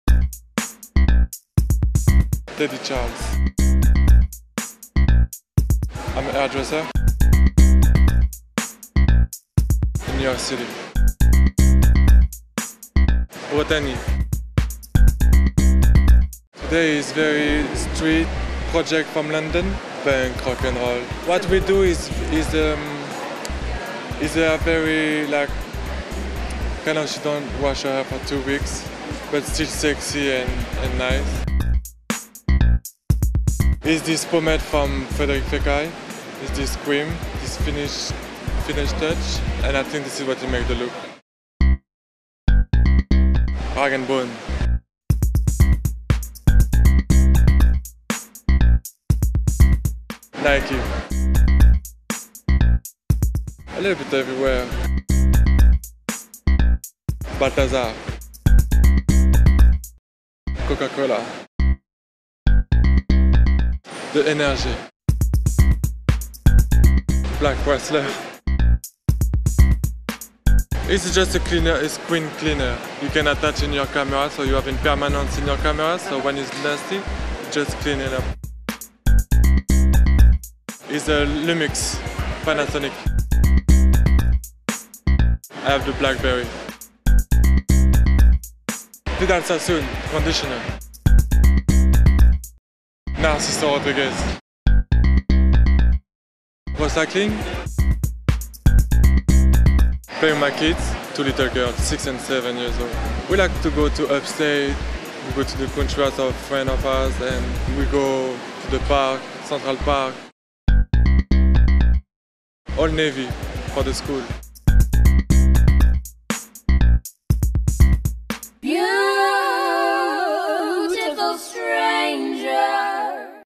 Teddy Charles - Hairstylist at Frederic Fekkai Salon
Spotted:  Backstage at the Rag & Bone fashion show

Beautiful Stranger shows up early for the Rag & Bone show to catch what's going on behind the scenes and finds hairstylist Teddy Charles for Frederic Fekkai creating the hair looks for the show, which he describes as "dirty in a sexy sort of way." In this vignette, the French-born stylist tells us Coke is the drink he can't do without.